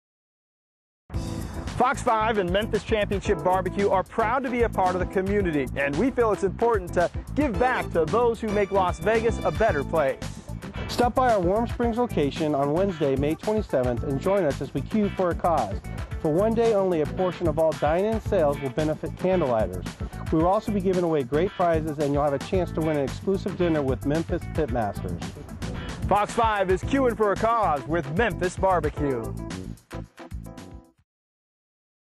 Memphis BBQ Charity Event
created to promote the Memphis BBQ Charity Event to benefit Candle Lighters